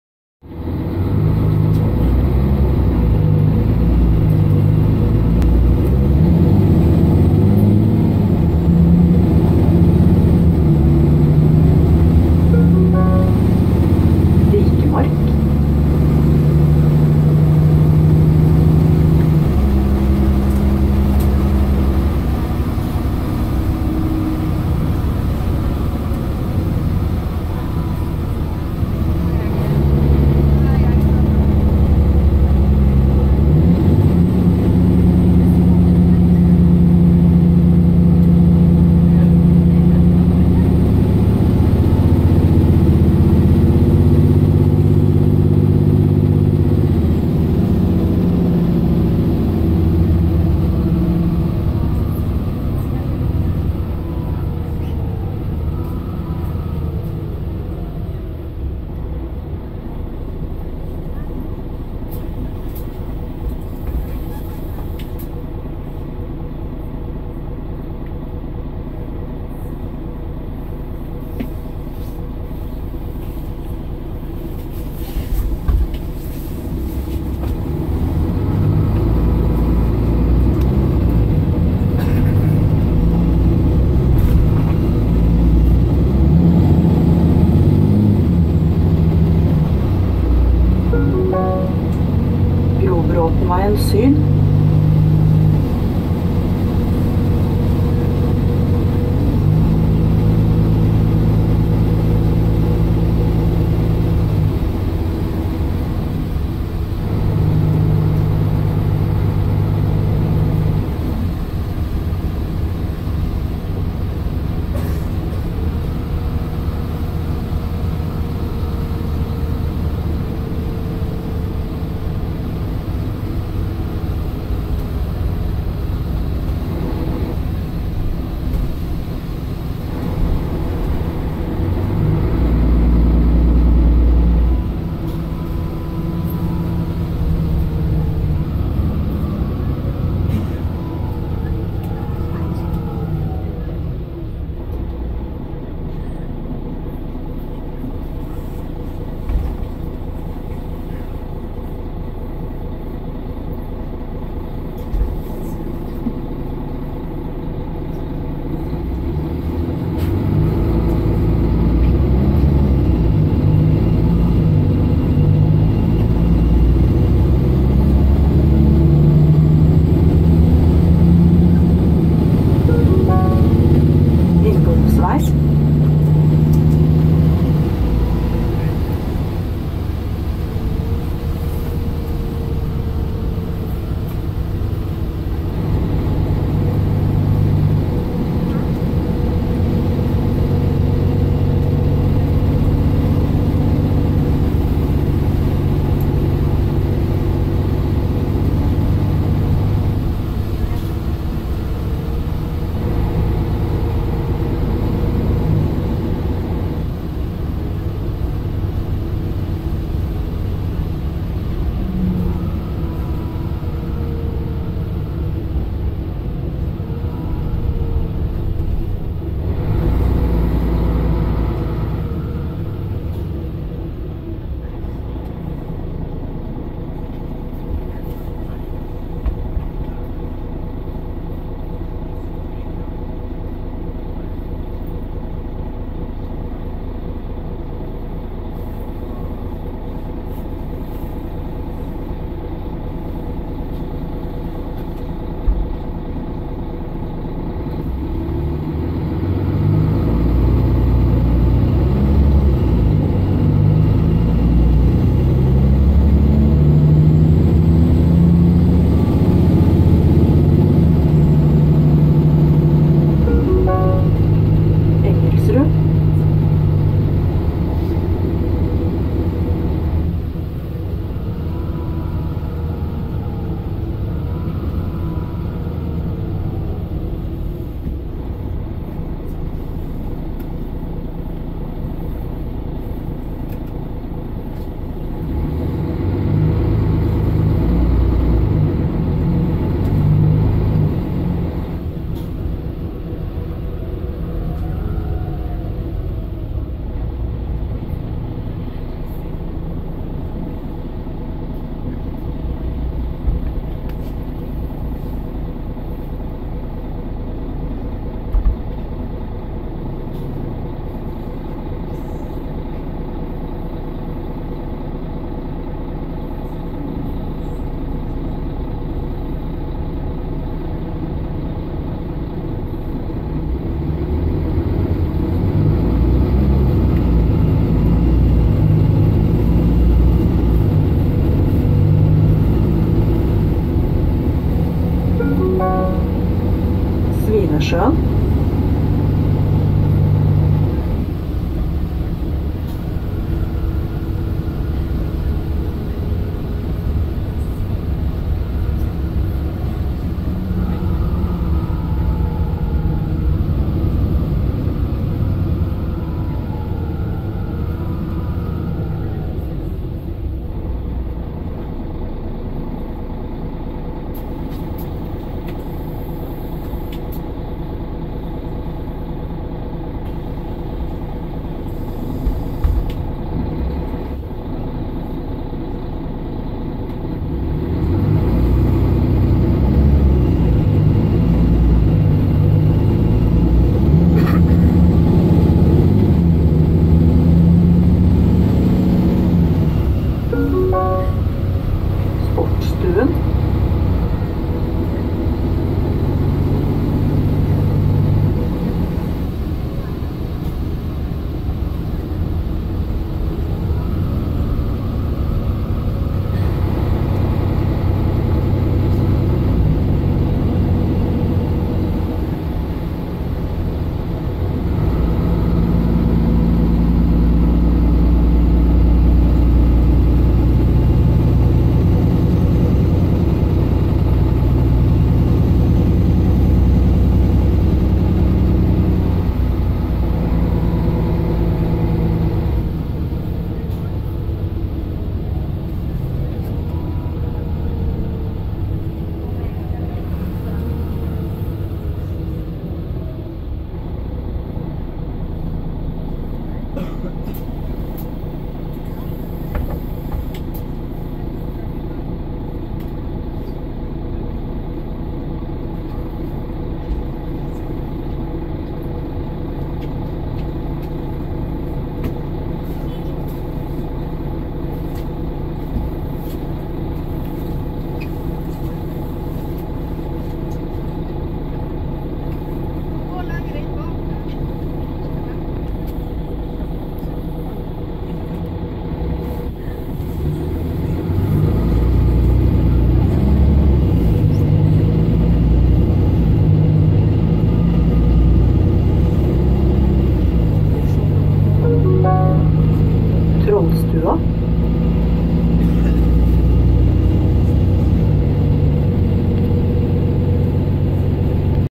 Mercedes Citaro o530 Full kickdown/Thrash *EXTREME KICKDOWNS*
Brilliant! absolutely astonishing, finally i got something good on video :D
Amazing kickdown's thrashing etc.
Bus- Mercedes-Benz Citaro o530 LE
Ecostandard - Bluetech 5/Euro 5
Engine - OM457hla/299 HP
220 KW/11967 cm3
12 liters
Transmission - ZF Ecolife 6 AP 1700B with overdrive
Weight - 12450 Kilograms
Bus - 8221
Route- 701